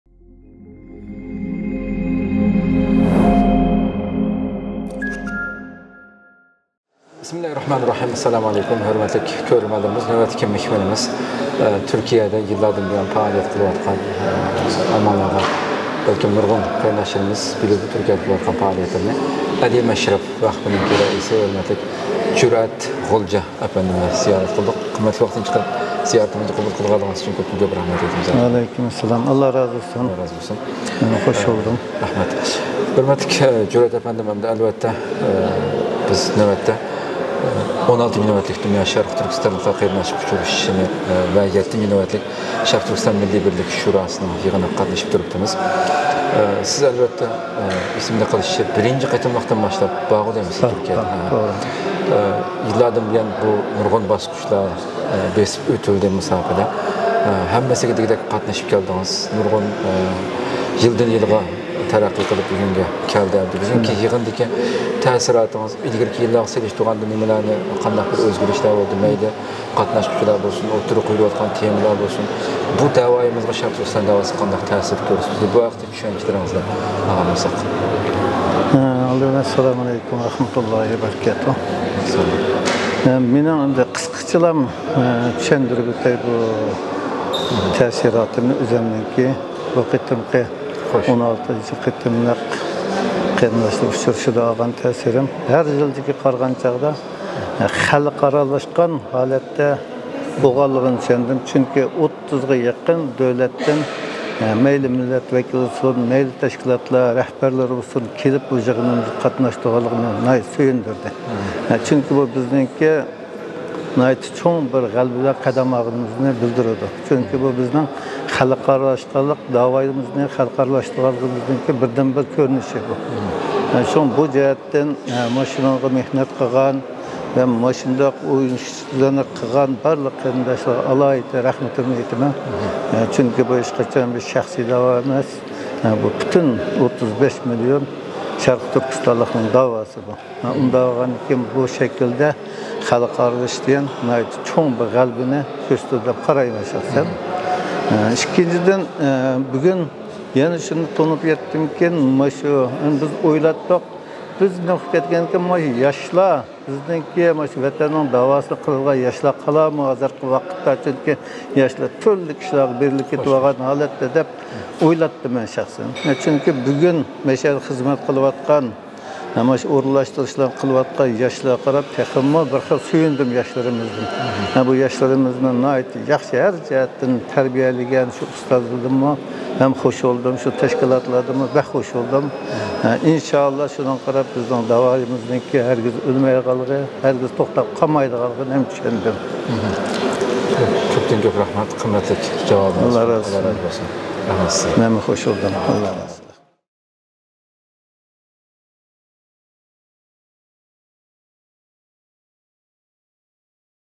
ئىلى مەشرەپ ۋەقفى رەئىسى جۈرئەت غۇلجا بىلەن قىسقا سۆھبەت
16-نۆۋەتلىك دۇنيا شەرقىي تۈركىستانلىقلار قېرىنداشلىق ئۇچرىشىشى ۋە 7-نۆۋەتلىك شەرقىي تۈركىستان مىللىي بىرلىك شۇراسى / ئىستانبۇل